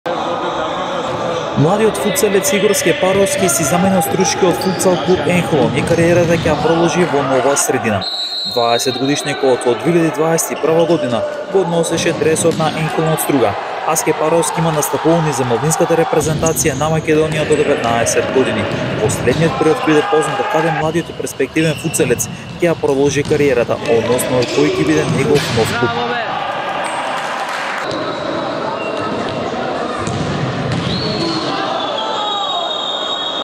Младиот футсалец Игор Скепаровски е слободен, кариерата ја продолжува во нова средина!
Младиот футсалец Игор Скепаровски си замина од струшките Енхалон, тој е слободен играч и кариерата ќе ја продолжи во нова средина!

Спортска Мрежа Podcast е посветена на македонскиот спорт каде особено медиумски простор ќе добиваат нисколигашите!